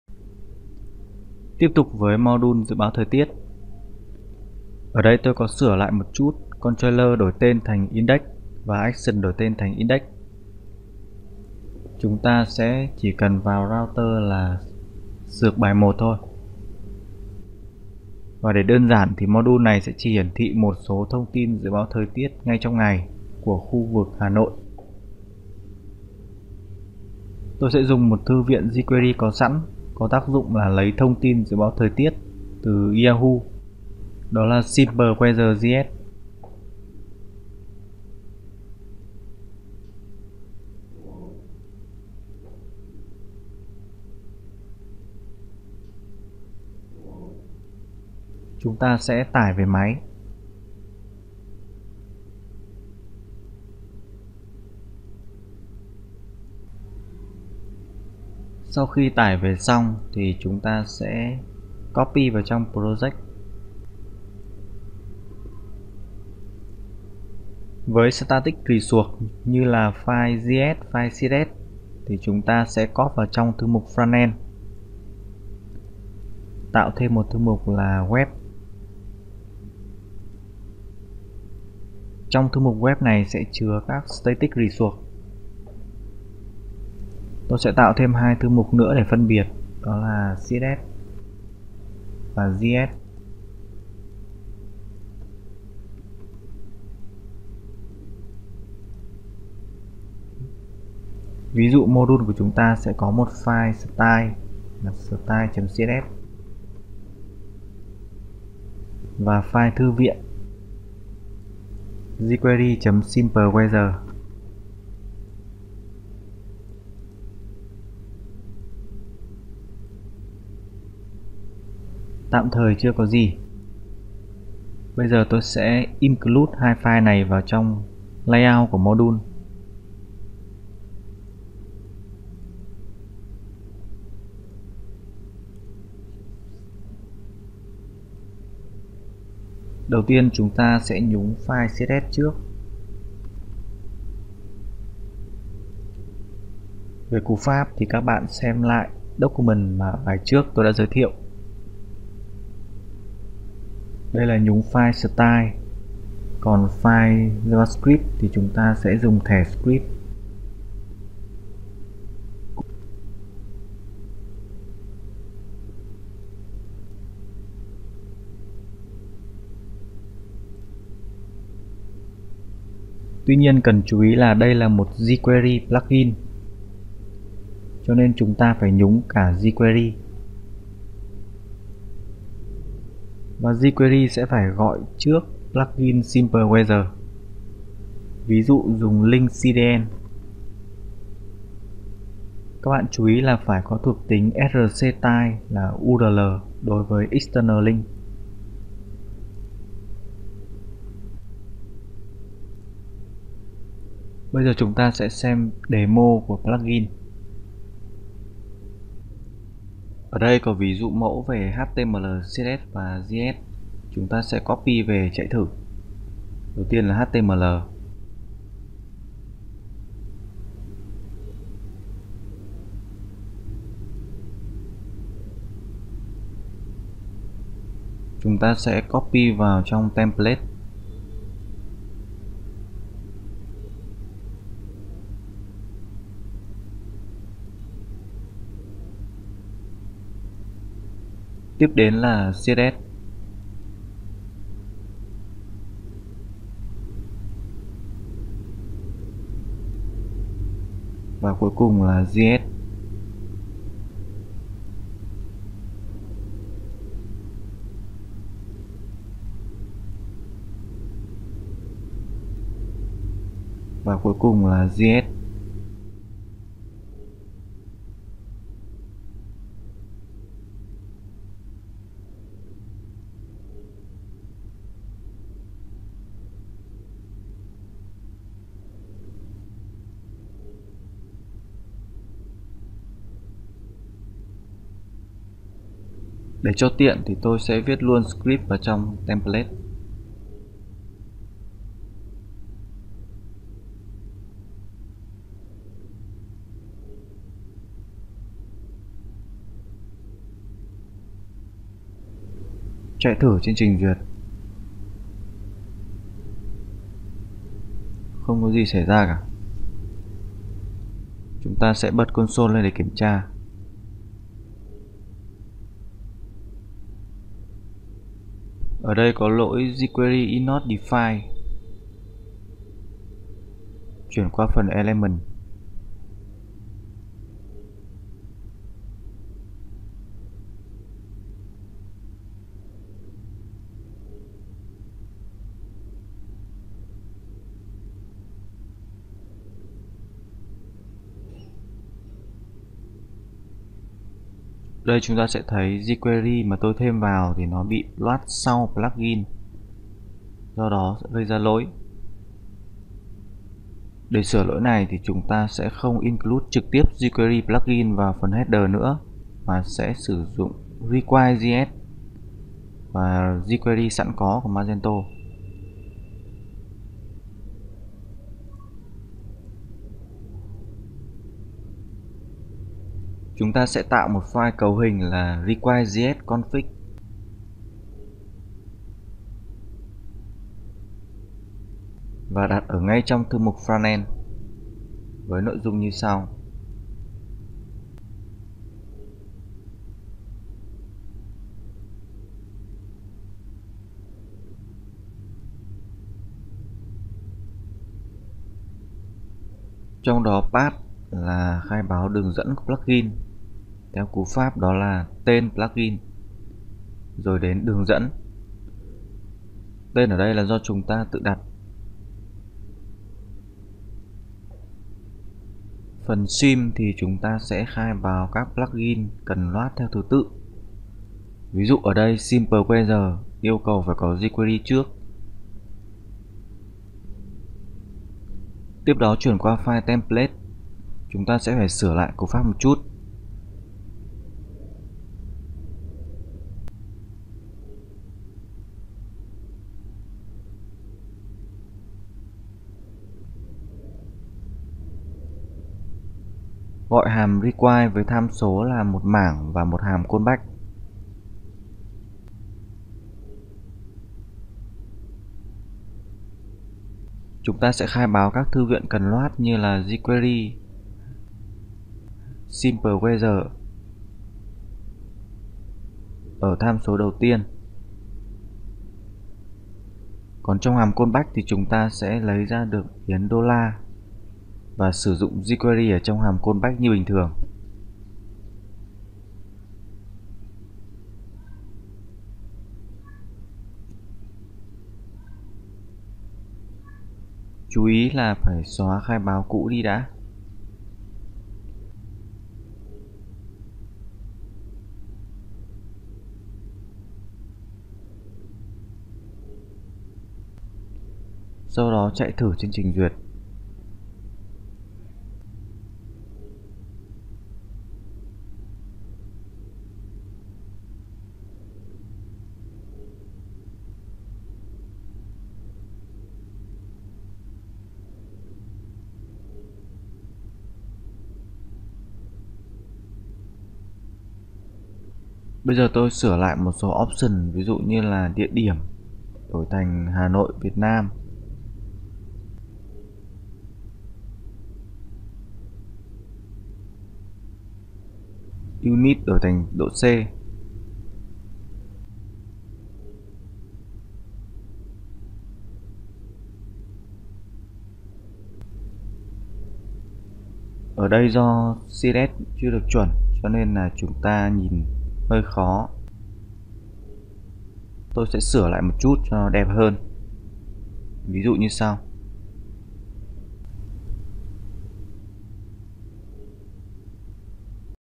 [Học Magento 2] Phần 2 Bài 4: Module dự báo thời tiết
- Khi khai báo đường dẫn trong requirejs-config thì không có đuôi .js ở cuối. Ví dụ với file myscript.js nằm trong thư mục web/js thì khai báo đường dẫn là Vendor_Module/js/myscript

- Trong 1 số trường hợp nếu bị lỗi requirejs thì có thể thử xóa thư mục pub/static/_requirejs đi và load lại module.

Tham khảo thêm các khóa học trực tuyến khác tại TechMaster với nội dung bao gồm cả Slide, Bài tập, Quiz, ...